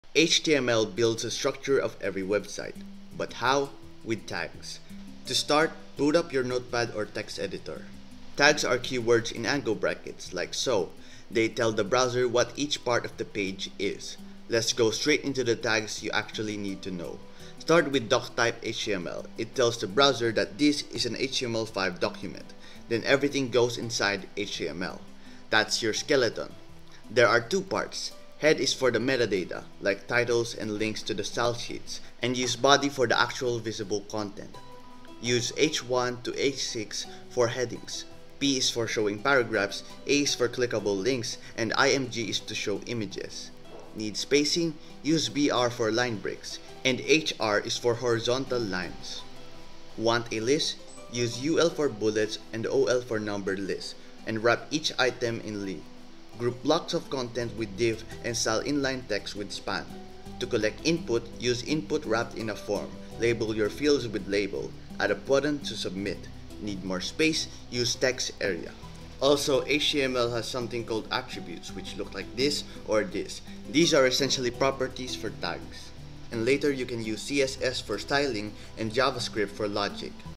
HTML explained in 80 seconds
I explain HTML and how to structure websites